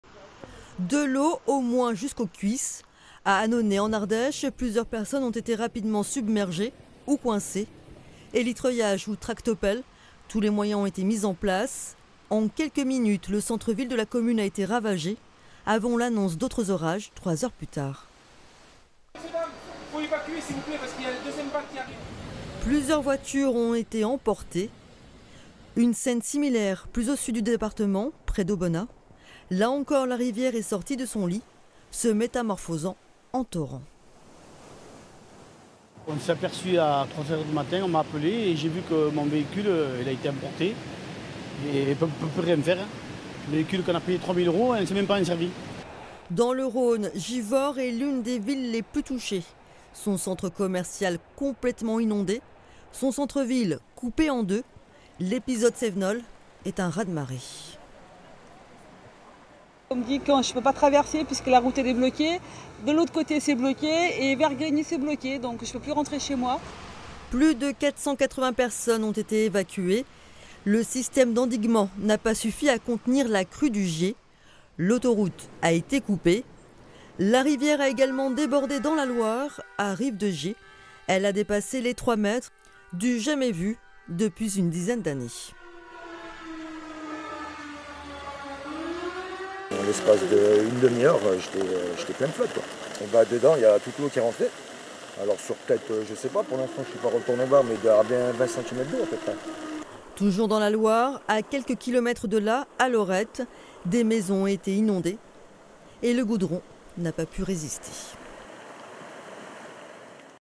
La Loire et l'Ardèche au lendemain des inondations
On n'est pas prêts d'oublier cette journée du 17 octobre 2024. La Loire et l'Ardèche ont été particulièrement touchées par les pluies diluviennes.
Retour sur les intempéries à Annonay dont le centre-ville a été ravagé, à Aubenas où la rivière est devenue torrent, à Rive-de-Gier où la rivière a dépassé les trois mètres.  Dans le Rhône, Givors a vu son centre commercial sous les eaux. L'A47 a été coupée.

Retrouvez-nous sur :
Notre site web